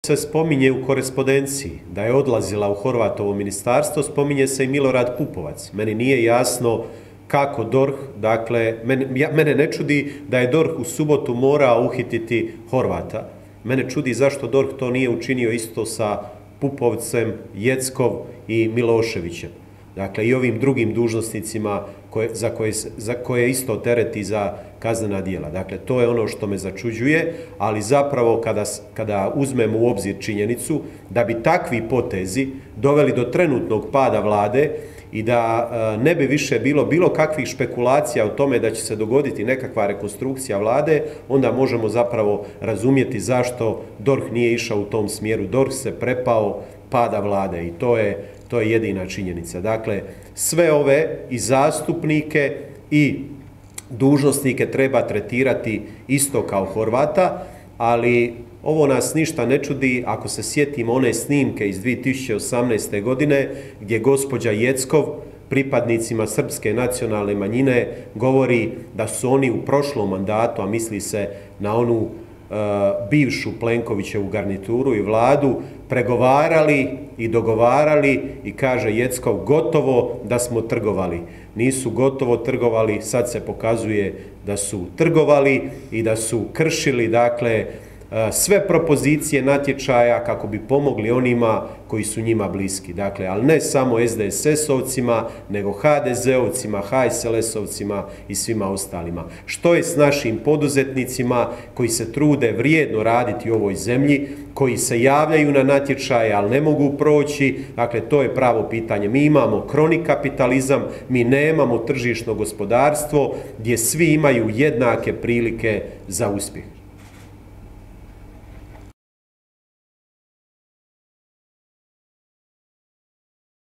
Grmoja: Zašto DORH nije uhitio Pupovca, Miloševića i Jeckov?
Na konferenciji za medije Kluba zastupnika Mosta održanoj u Saboru o zahtjevu za raspuštanjem Sabora govorili su zastupnici Božo Petrov, Nikola Grmoja, Marija Selak Raspudić, Nino Raspudić i Zvonimir Troskot.

VIDEO: Matrik Macek/PIXSELL
Photo gallery: